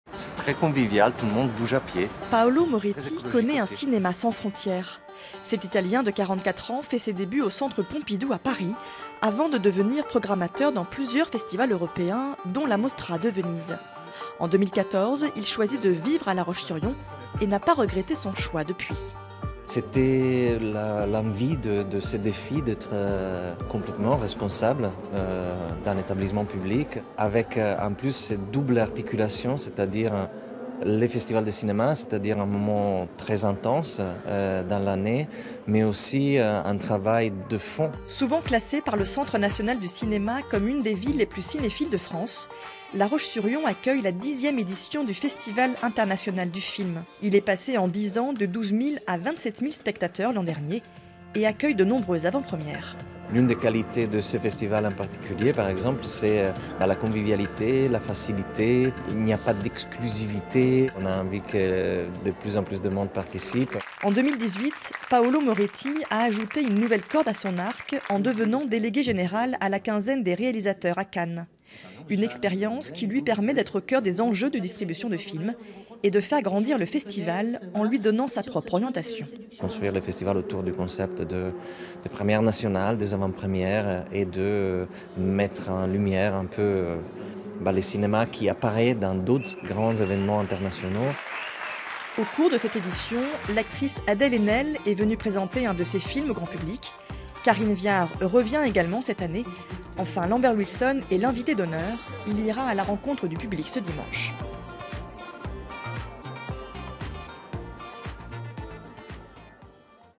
Vendée : Paolo Moretti, chef d'orchestre du Festival International du Film
Le FIF, Festival International du Film de La Roche-sur-Yon fête ses 10 ans. Des dizaines de films en avant première, plus de 250 projections, des invités... et à la baguette Paolo Moretti. C'est le délégué général de ce festival.

F Borius, D. Raveleau, N. LAMBERT, N. SALIOU-TENDRON